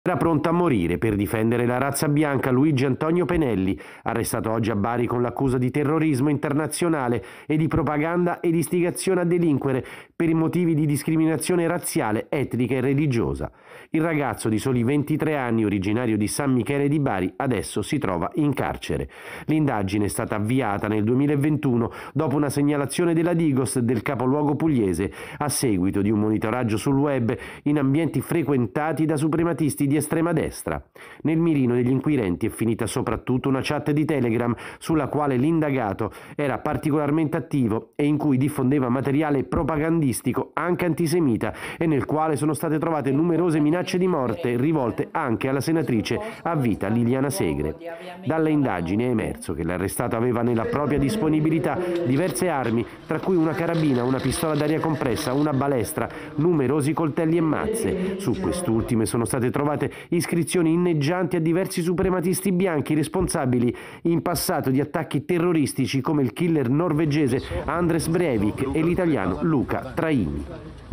Arrestato in Puglia 'lupo solitario', “pronto a morire per la razza bianca”
Arrestato in Puglia un giovane "lupo solitario", appartenente ad un gruppo di suprematisti bianchi. Servizio di Federico Plotti